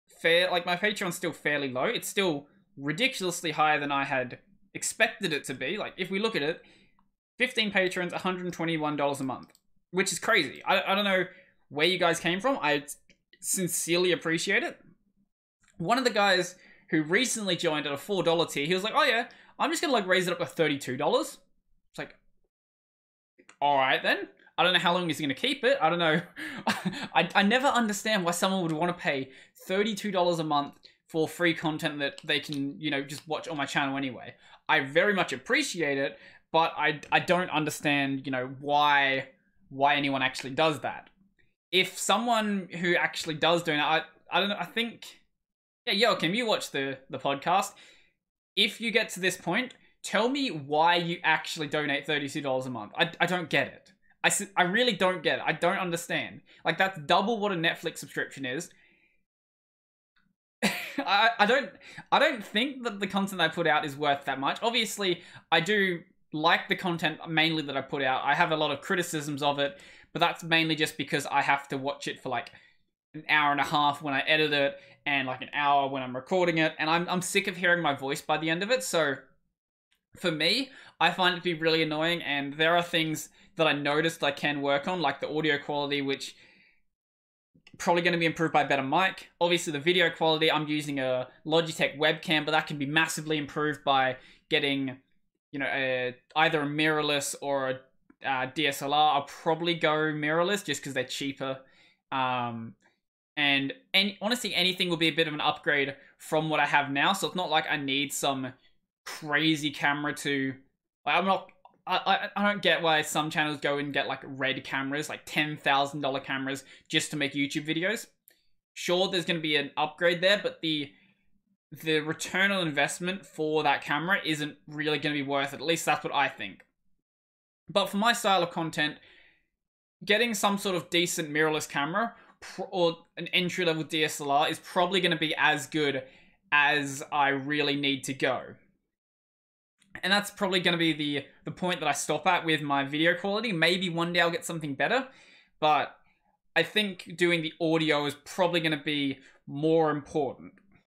Receiving Donations Is Weird - Solo Clip
On today's Tech Over Tea Clip I discuss how weird it is to receive for the work you put out into the world and also how confusing I find it that I have some people donate insane amounts like $32 a month.

► BTC Wallet Address: 1Aokiv3pFQXUEmh2LbzZQAwxMvq6bpT2UN
► ETH Wallet Address: 0x80451867c86bdf08c3888d407c1e3fcb6add61ed
► LBC Wallet Address: bLRN9fm17sCexKfgbYqmMj5xskZF2ogpEh